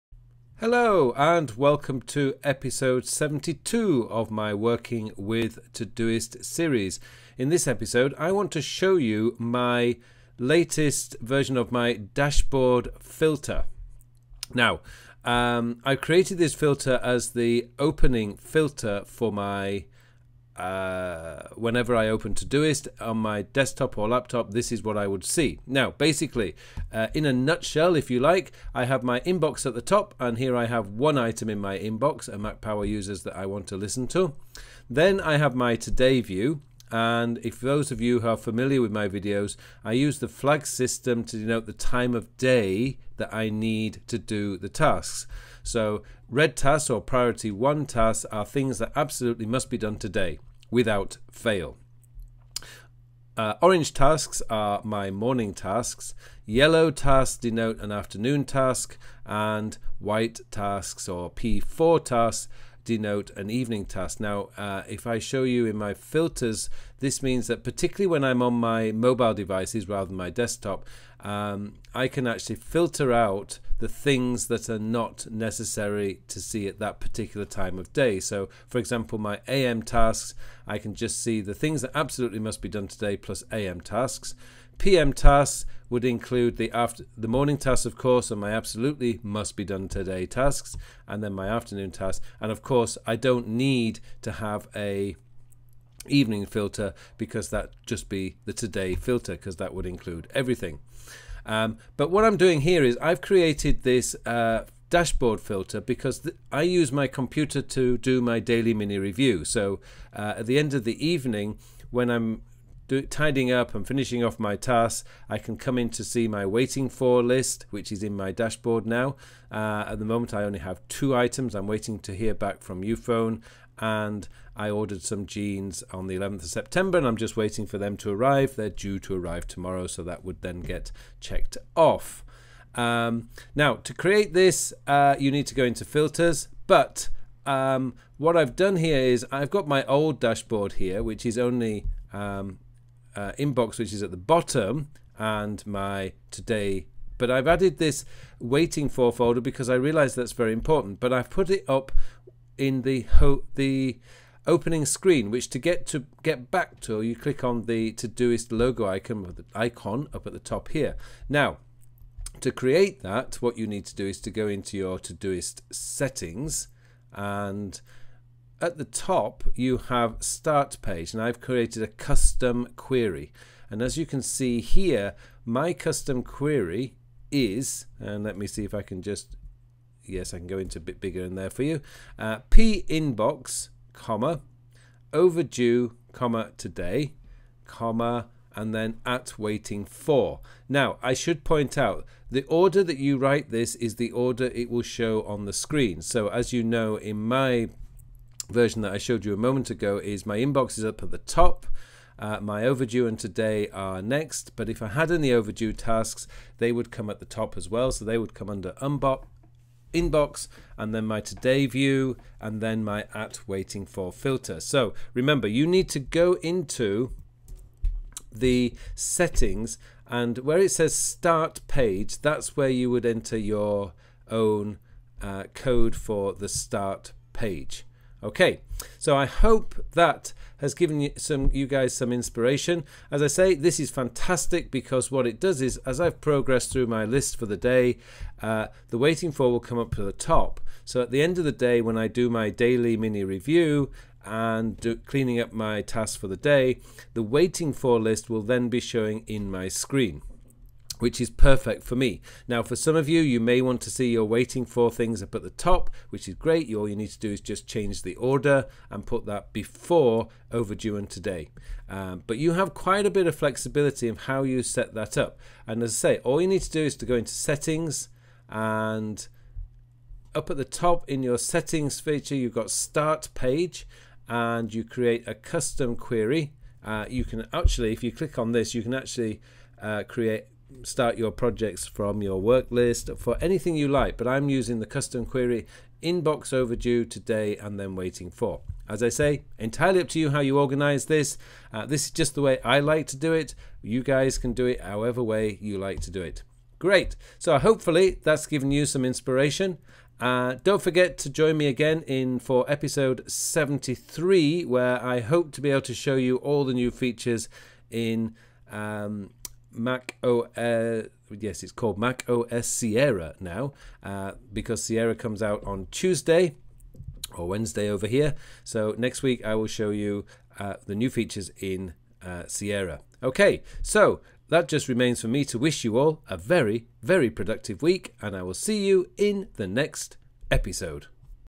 Working With Todoist | Ep 72 | My Latest Dashboard View
In this episode I show you my latest dashboard view and how and why I created it.